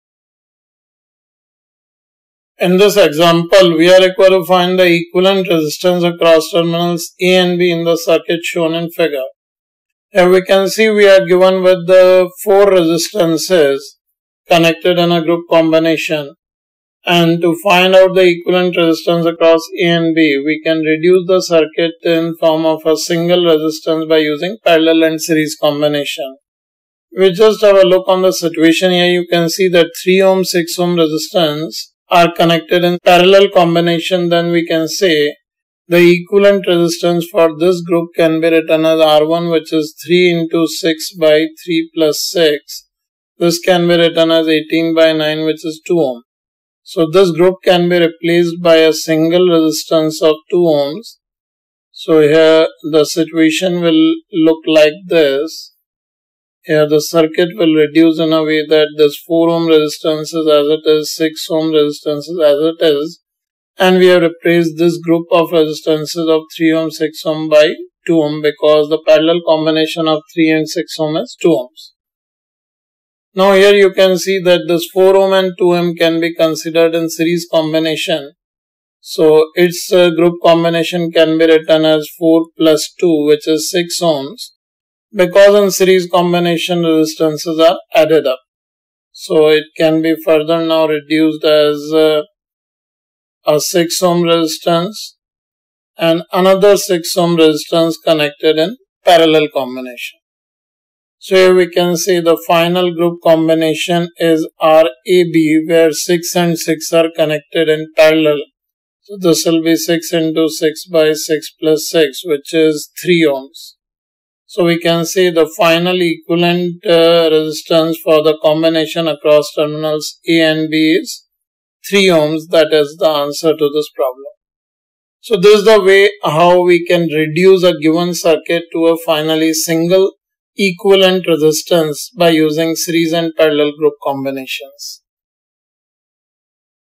Class 12 Physics | Current Electricity | #24 Solved Example-9 on Current Electricity |For JEE & NEET
PG Concept Video | Current Electricity | Solved Example-9 on Current Electricity by Ashish Arora
Students can watch all concept videos of class 12 Current Electricity for jee & neet in proper sequence through PG channel playlist.